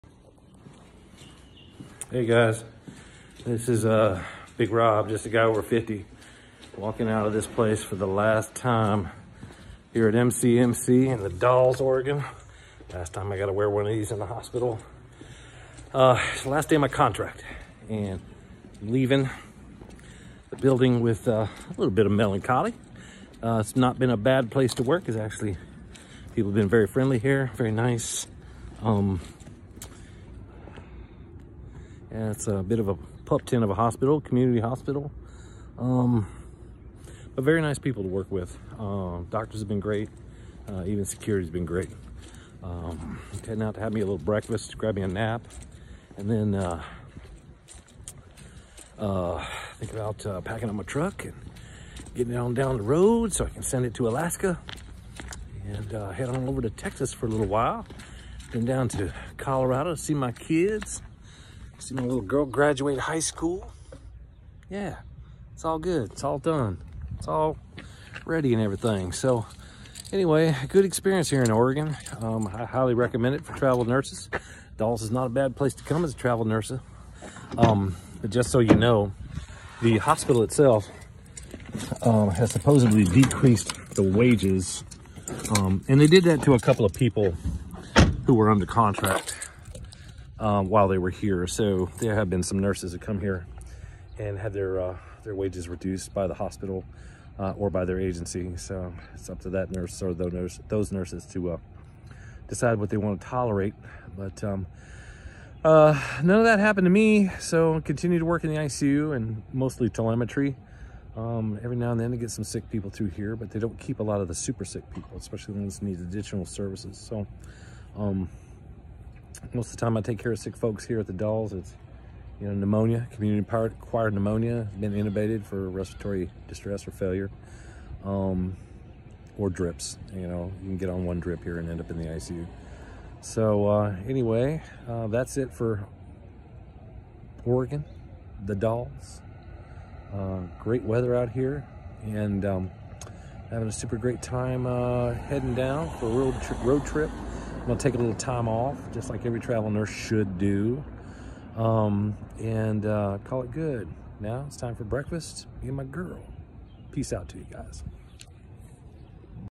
Big Rob last days in The Dalles, OR
So I finished up my last shift at MCMC in the ICU but mostly tele.  The Dalles has been really good to me.  I’ve met a lot of nice people in this tiny community of hard-working folks. I finish my contract the one I started in January and I’ve had a good time exploring Oregon trying to do it as cheaply as possible to the beach into the harbors been all over the western Seaboard as far south is Florence as far north as a Astoria.  I’ve enjoyed every minute of it now it’s time to take some time off all travel nurses should do that head back to Alaska and enjoy my house and the great weather and some grilling and some fishing piece out guys